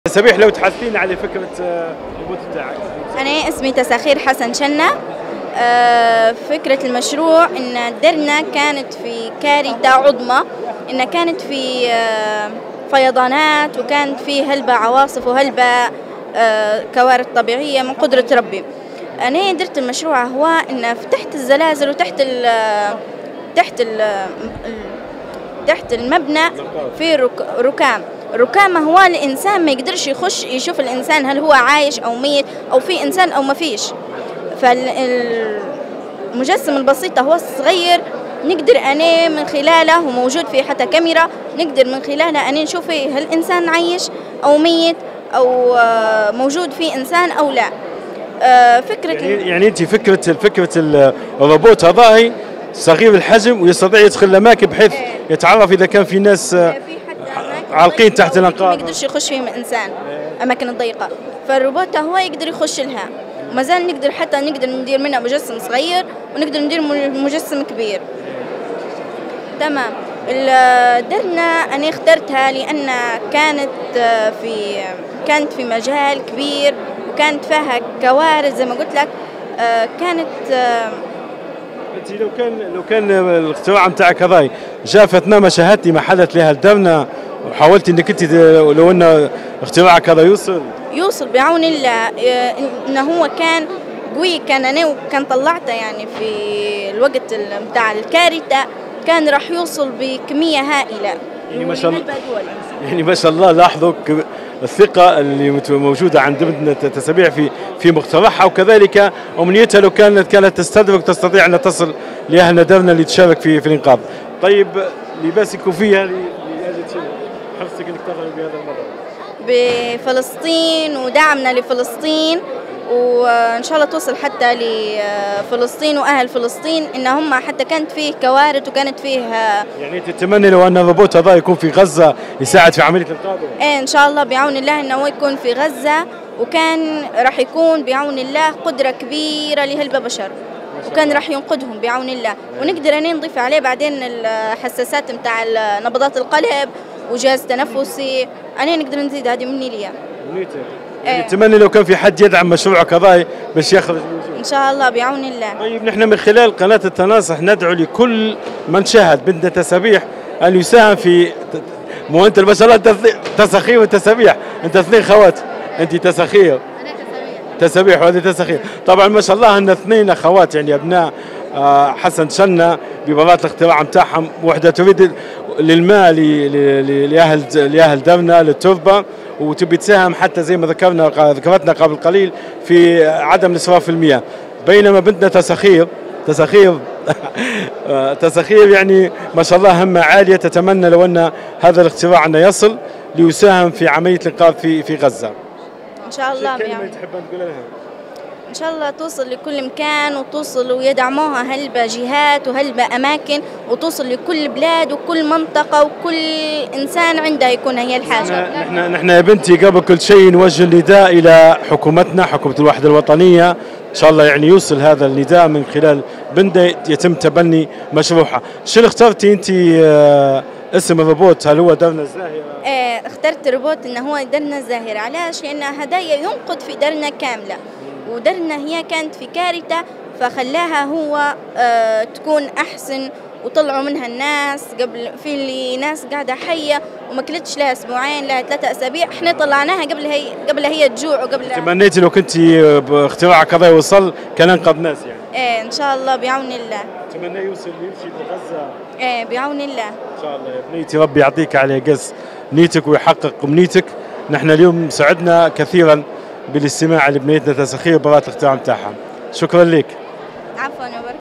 تساخير شنة: أحد المشاركين في معرض إنماء للمواهب الشابة المقام بالقبة الفلكية طرابلس
تساخير شنة/ عضو نادي بداية الانطلاقة للروبوتات
أحد المشاركين في معرض إنماء للمواهب الشابة المقام بالقبة الفلكية طرابلس والذي سلط الضوء على القضية الفلسطينية ومدينة درنة.

ــــــــــــــــــــــــــــــــــــــــــــــــــــــــــــــــــــــــــ
ــــــــــــــــــــــــــــــــــــــــــــــــــــــــــــــــــــــــــ
يمكنكم تحميل تطبيقنا TANASUH TV على الهواتف الذكية: